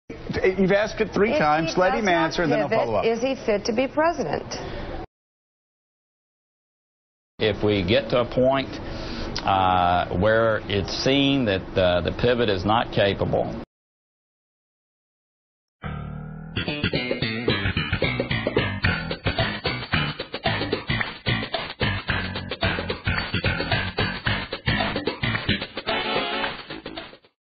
The Pivot
This was my first attempt at video. When I heard this exchange between Mika Brzezinski and Senator Bob Corker when they were discussing Trump, I couldn't stop laughing. All I could hear was George from Seinfeld. Since when is a Pivot a noun? Just a silly thing.